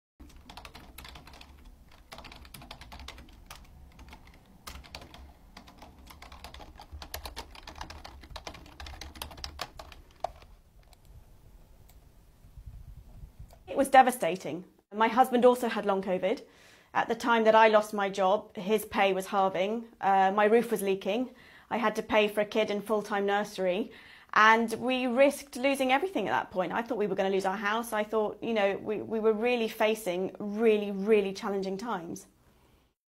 'We risked losing everything,' says GP who contracted COVID on the frontline
The British Medical Association (BMA) has found one in five doctors with long COVID have been forced to stop work or significantly cut back on their hours. 🩺

Dr Amy Small spent six months unable to work at all with long COVID and lost her GP partnership in the process.